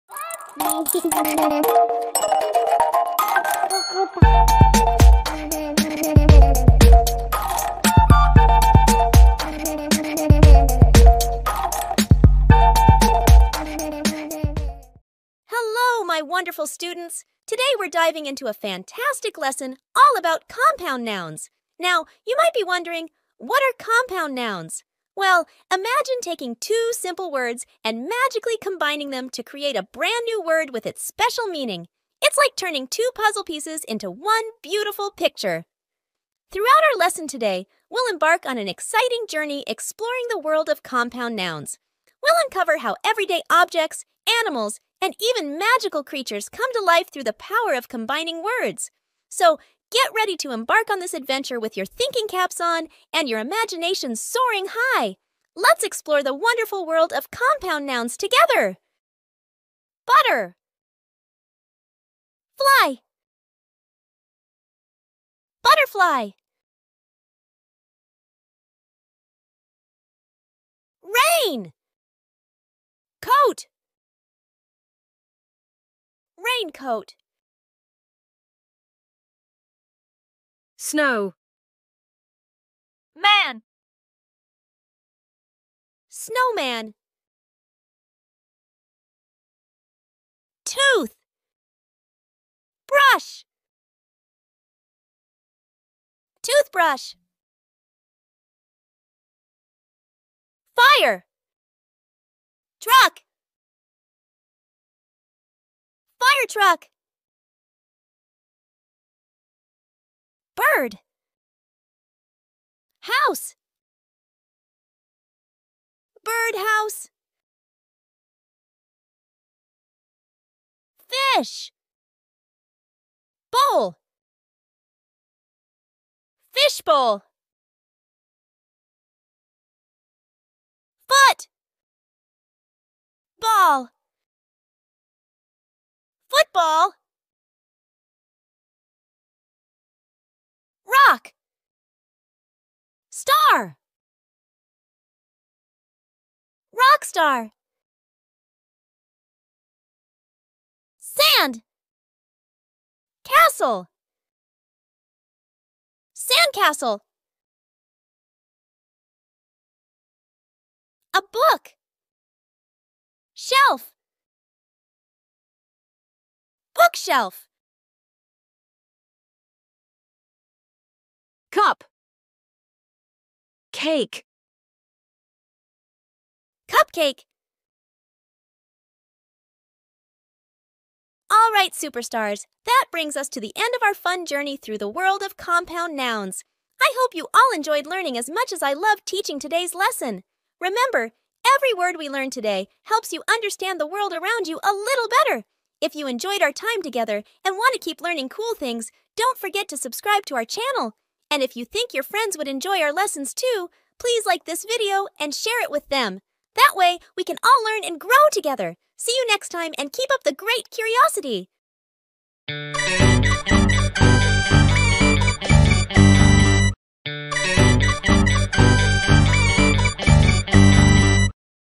Compound Nouns for Kids 📚✨ | Fun English Learning for Beginners
🌟 Welcome to our exciting world of Compound Nouns! 🌟

In this fun and educational video, join us as we explore the magic of combining words to create brand-new ones. 🎩✨ Get ready to embark on a journey filled with laughter, learning, and lots of creativity! 🚀📚

🔍 What you'll discover in this video:
🔹 What are compound nouns and how do they work?
🔹 Fun examples of compound nouns that you use every day!
🔹 How combining words can unlock endless possibilities!

So, grab your favorite snacks 🍿, get cozy, and let's dive into the fascinating world of language together! 🌈📝

Let's spread the joy of learning far and wide! 🌟

WOOHOO! You’re in the club! 🎉

🔔 Your support helps us create more amazing videos for you!